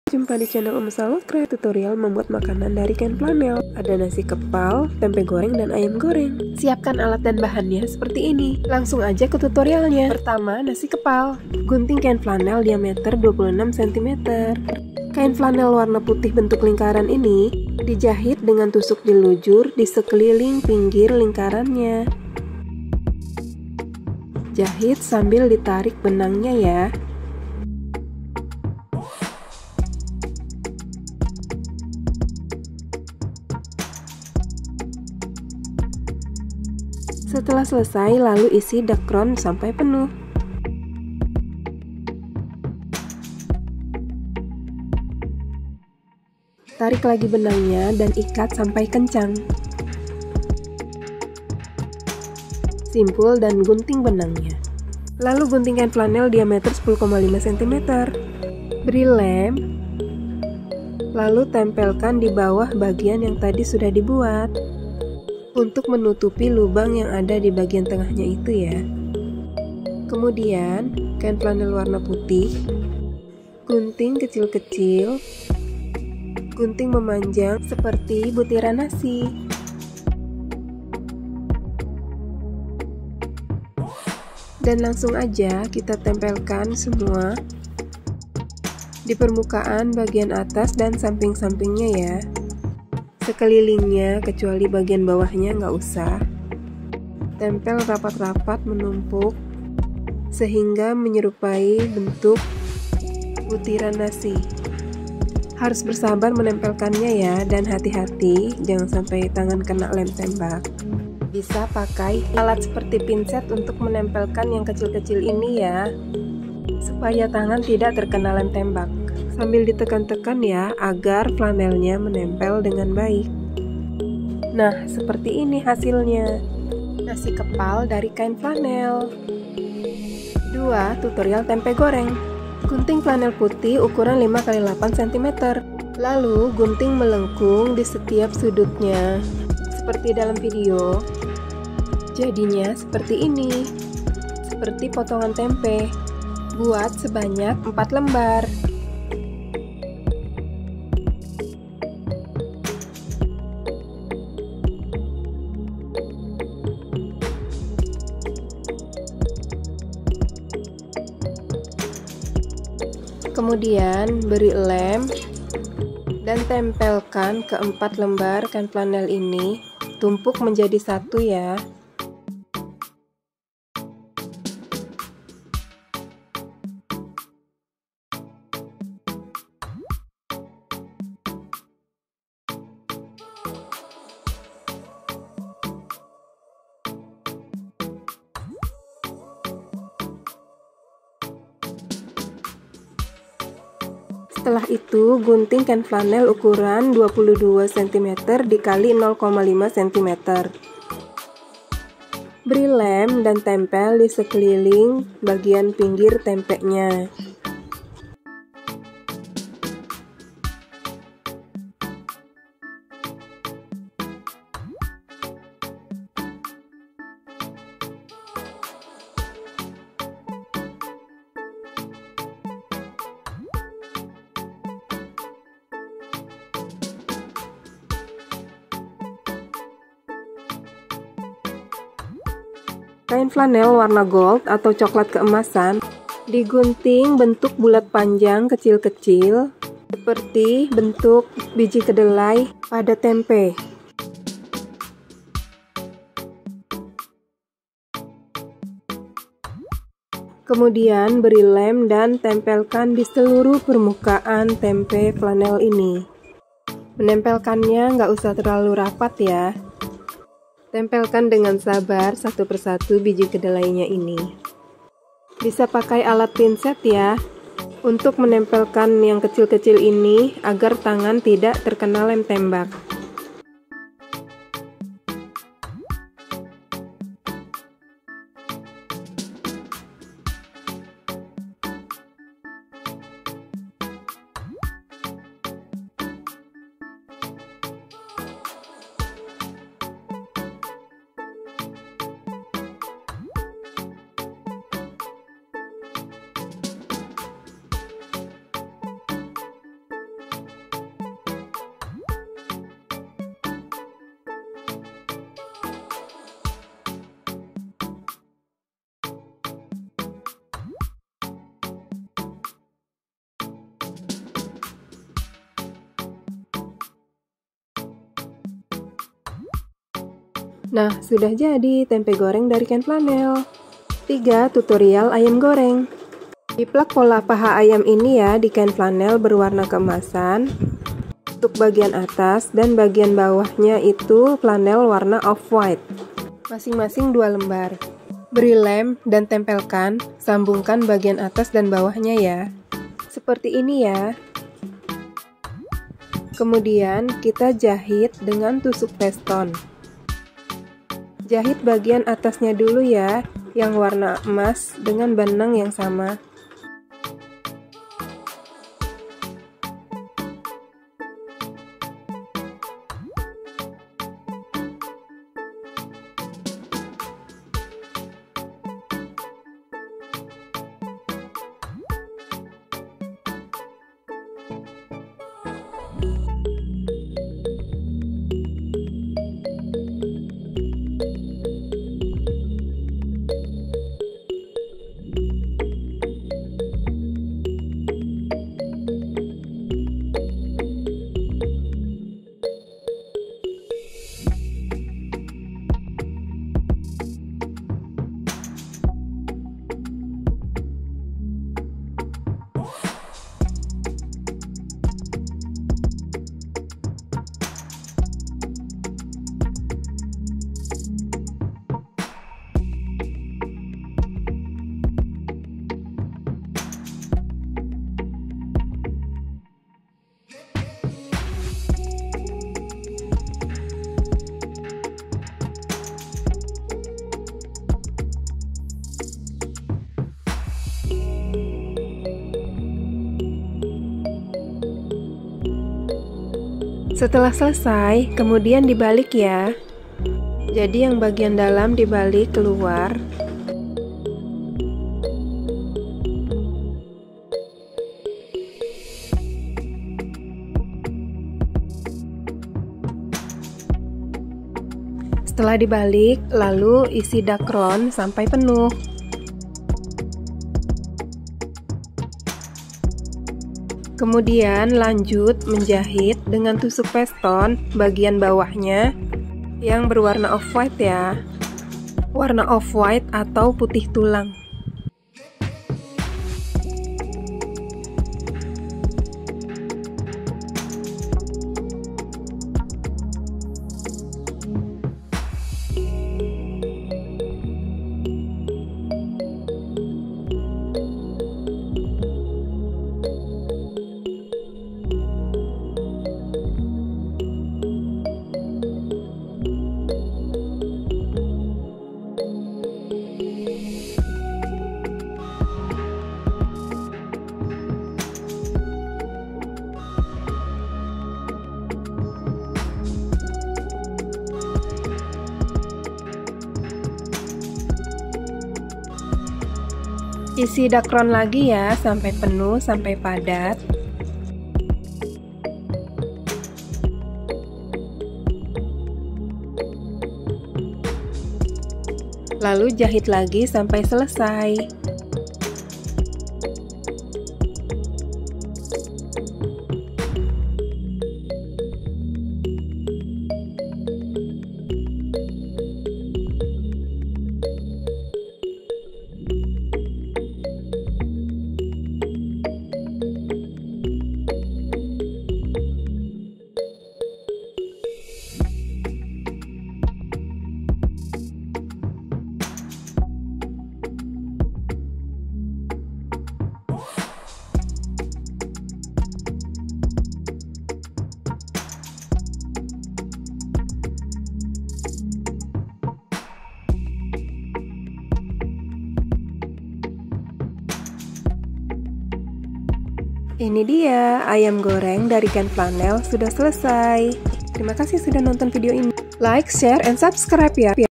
DIY felt food||tutorial makanan flanel - cara membuat nasi, tempe dan ayam goreng dari kain flanel
Assalamualaikum, kali ini Ummu Salwa Craft membuat video tutorial makanan dari kain flanel, yaitu cara membuat nasi kepal, tempe goreng dan ayam goreng. Makanan flanel ini bisa digunakan sebagai alat peraga pendidikan, bisa juga untuk mainan anak . Selamat menonton video nya, semoga bermanfaat.